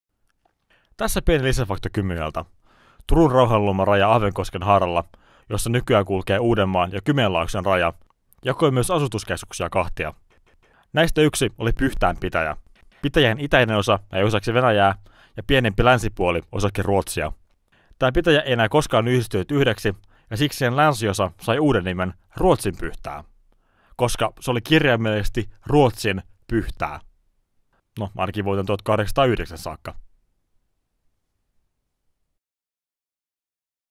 †Lisätieto: Ruotsinpyhtää // †Footnote: Ruotsinpyhtää, 'The Swedish Pyhtää"
Pieni lisävideo siitä, miksi niinkin idässä kuin Kymeenlaakson rajalla on Ruotsiin viittaava paikannimi //
A short additional video about how as far East as Kymeenlaakso there is a place named after Sweden.